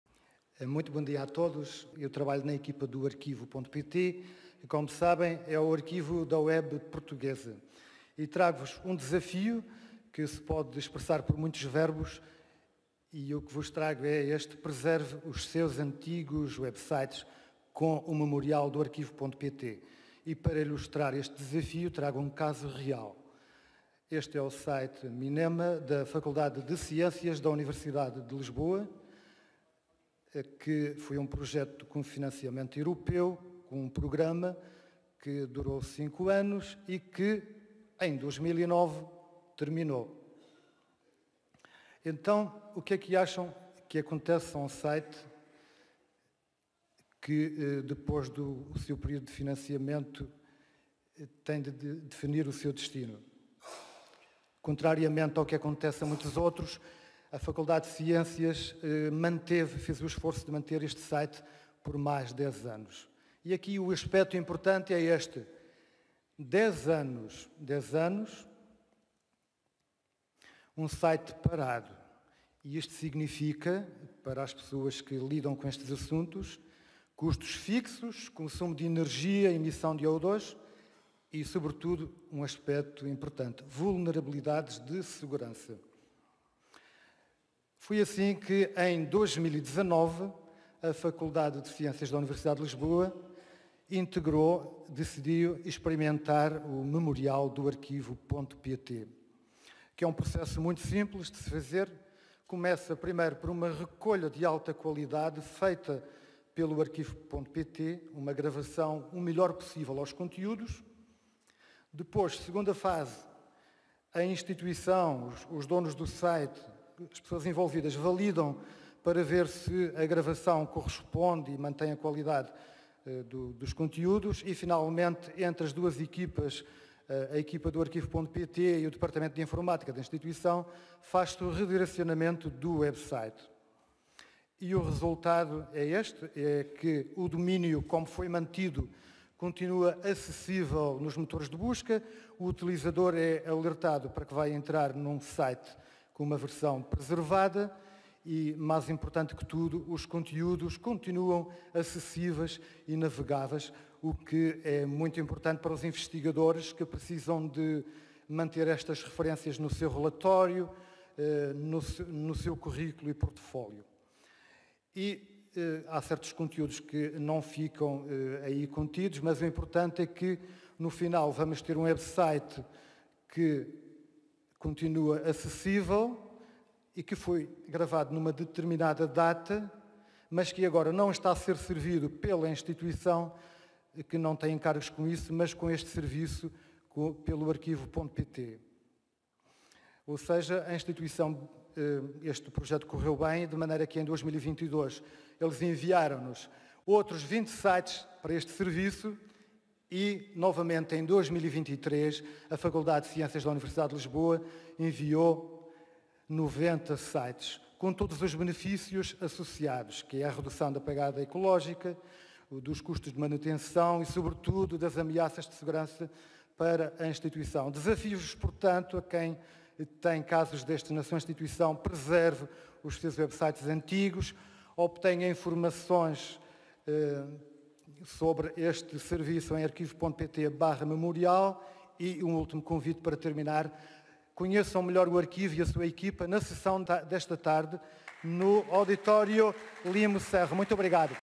Preserve os seus antigos websites com o Memorial do Arquivo.pt. Jornadas FCCN 2023
Apresentação do serviço Memorial do Arquivo.pt na sessão "Zapping da FCCN", durante as Jornadas FCCN 2023 realizadas na Escola Naval, em Almada, entre 27 e 29 de junho.